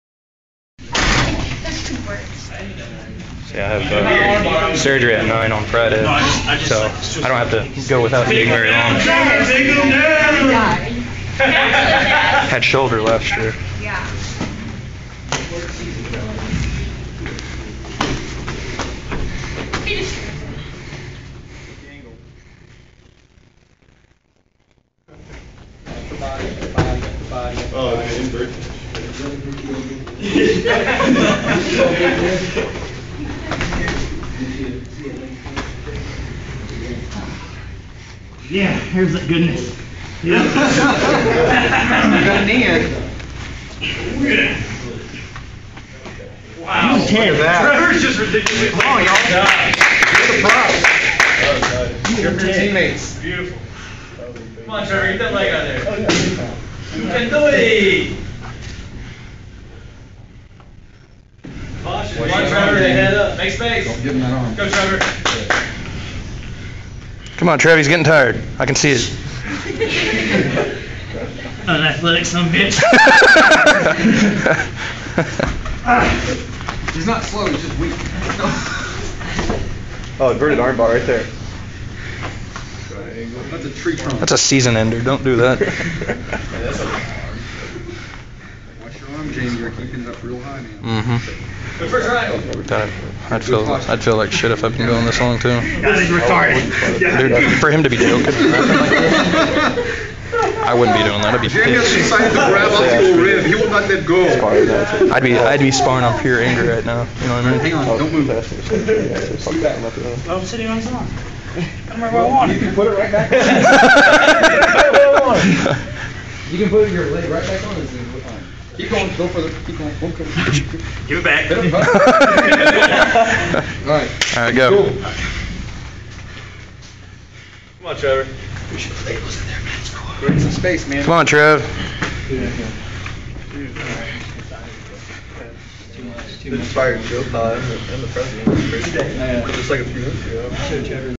Trevor vs Big Jamie Blatnick (part 1)
Trevor sparring Jamie Blatnick as part of his blue belt journey.

Camera went off half way through is the reason this match is in 2 parts